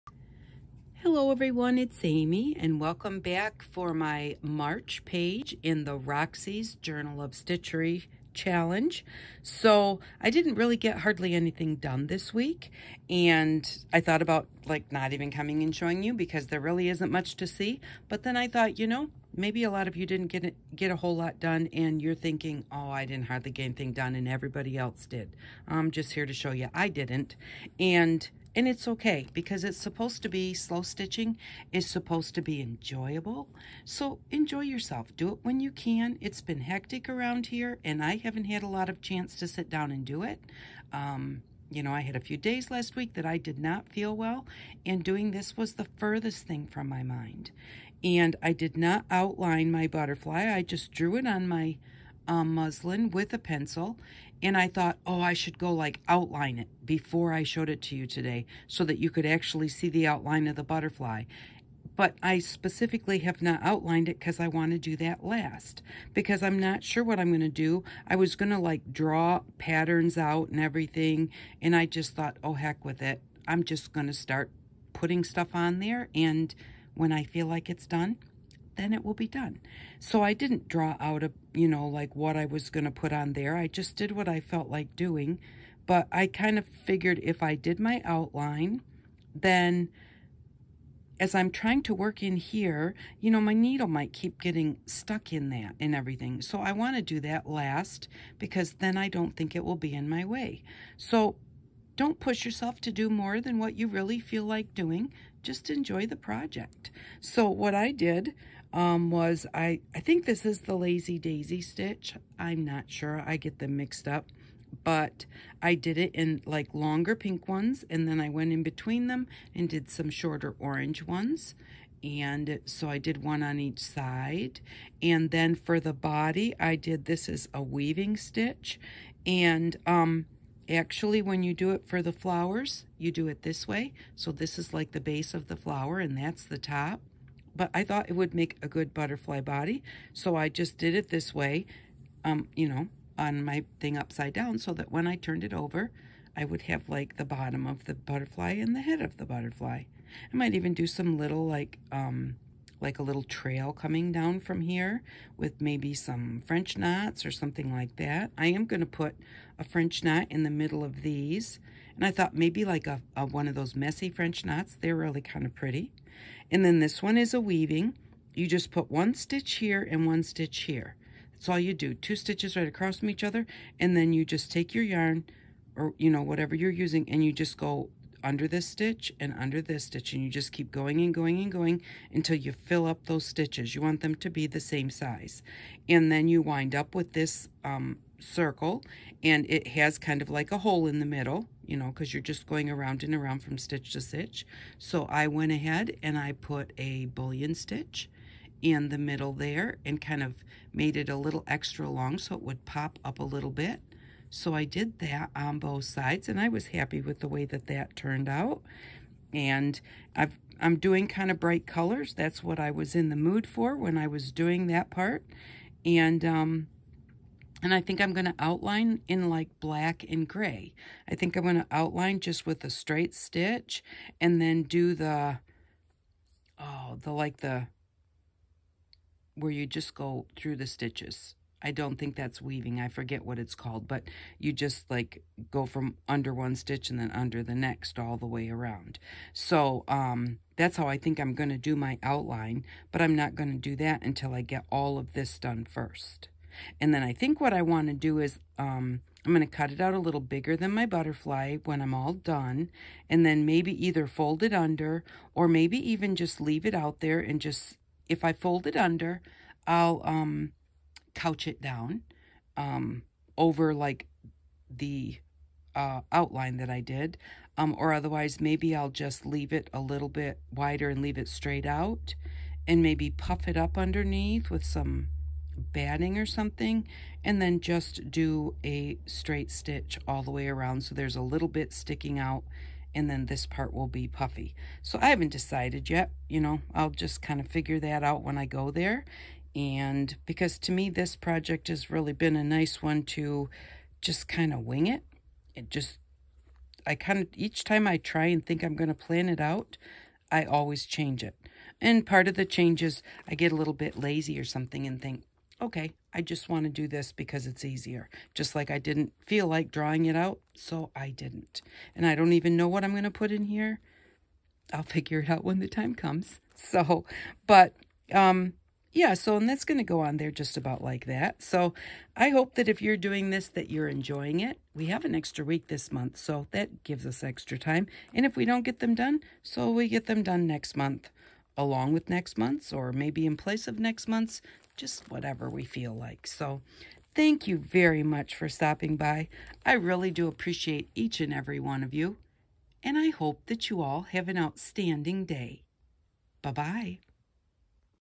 roxysjournalofstitchery Journal of Stitchery, video 12, March block, little bit on the Butterfly :)
I am going to create along with Rachel and Sarah for this fun challenge :)  I hope everyone joining in has as much fun as I have had already :)  tfw, and I hope you all have an Outstanding Day :) amy